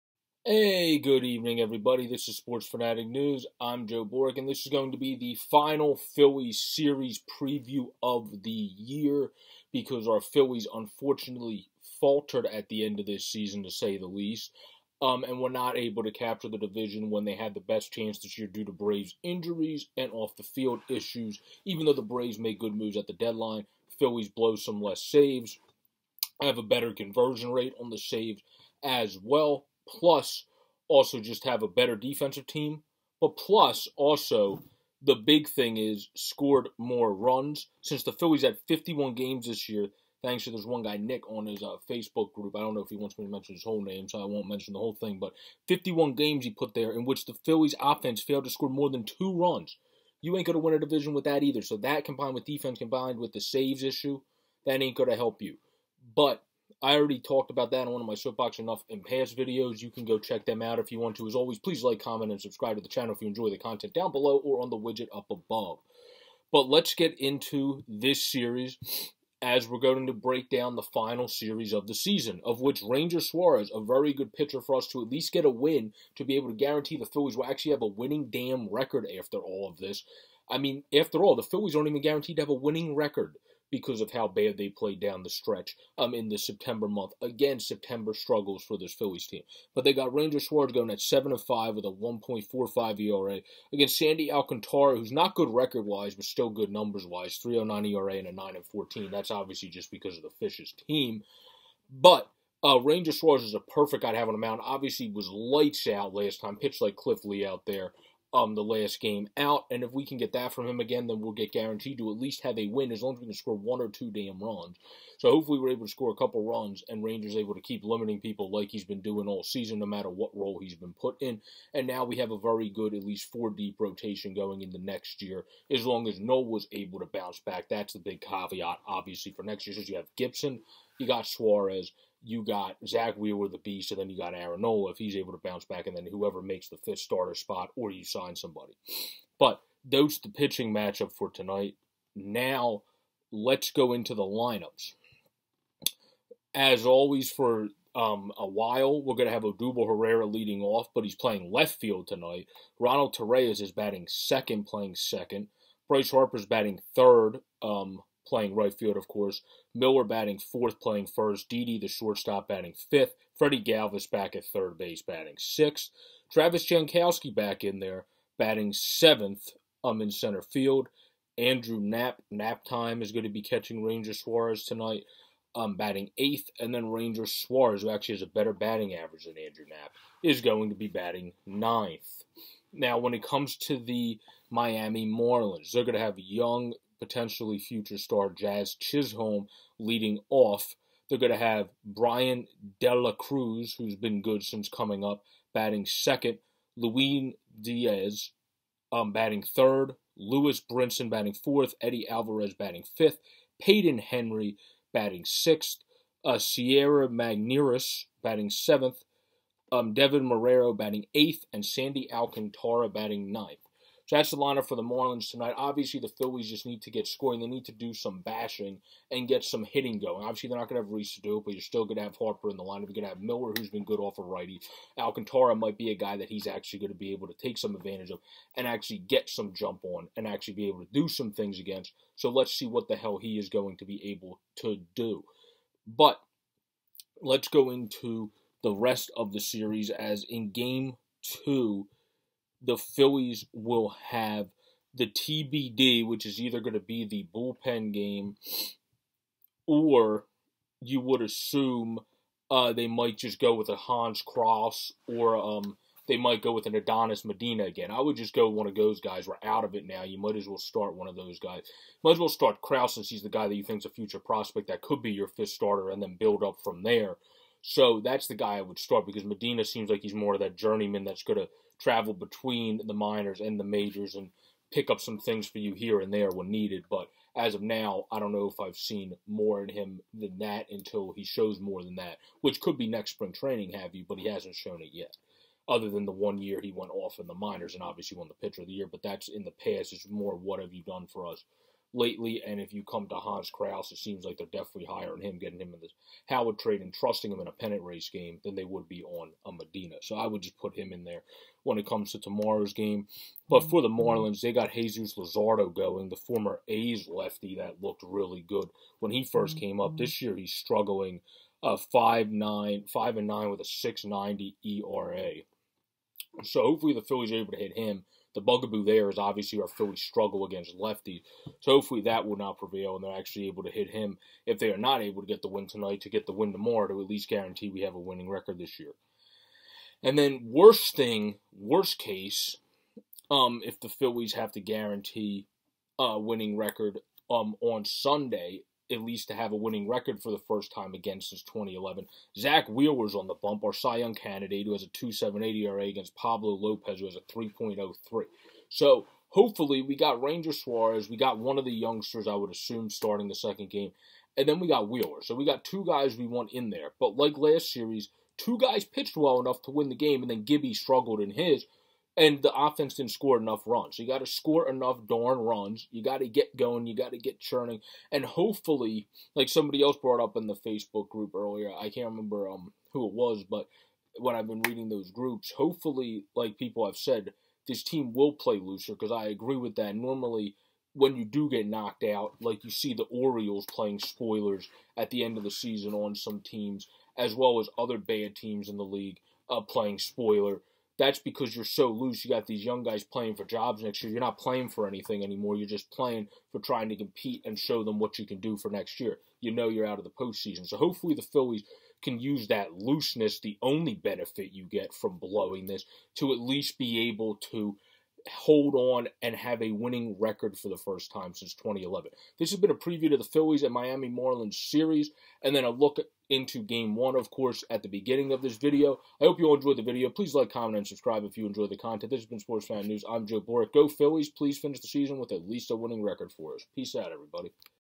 Phillies Vs Marlins Series Preview
In the final series preview of the year, after our Phillies have another September collapse Pro Joe breaks down the matchups for each game and dives into how the Phillies can avoid an even worse fate. After all, this not even having a winning record. The offense needs to show up 51 games of 2 runs or less is unacceptable.  Time to show up now to at least keep the winning record. If you enjoy the content please like and sub your support is appreciated. Go Phillies Beat the Marlins in at least one please to claim a winning record.